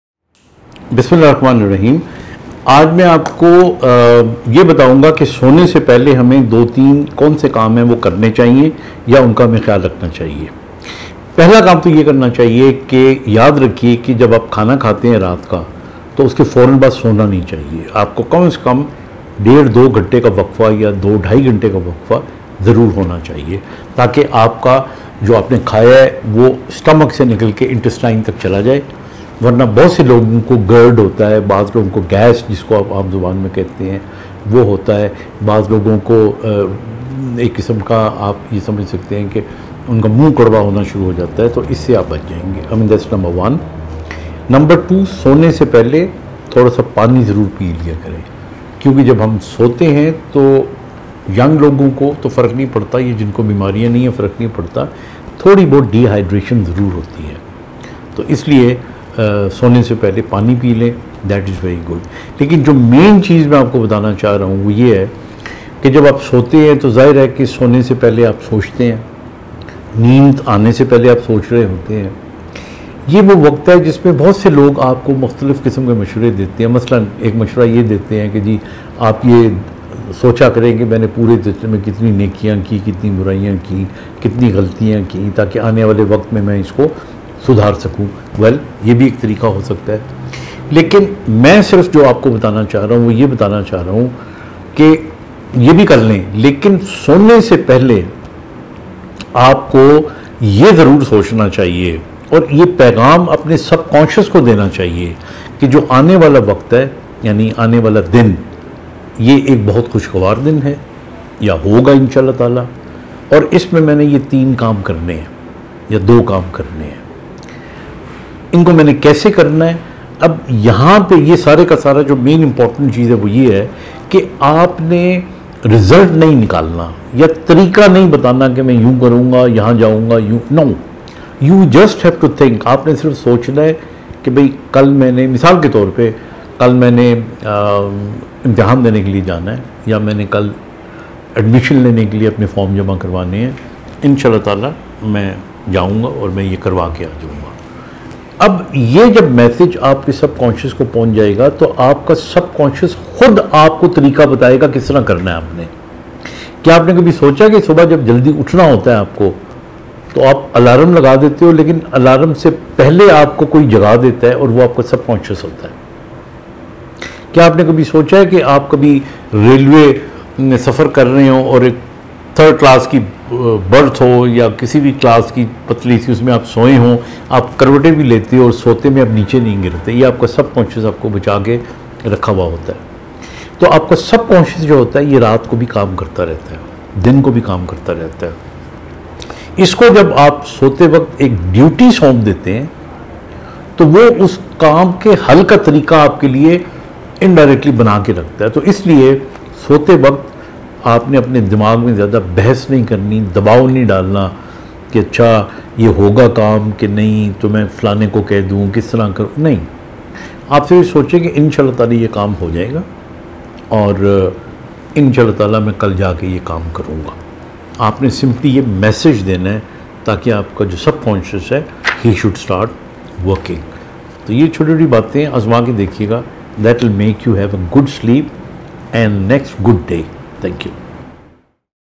Do These 3 Things Before Sleeping - By Dr. Khalid Jamil
Dr. Khalid Jameel Akhter (Neurologist ) discussing '' 3 Things Before Sleeping '' Happy Lifestyle
رات کو سونے سے پہلے یہ 3 کام لازمی کریں؟ صحت مند زندگی کا راز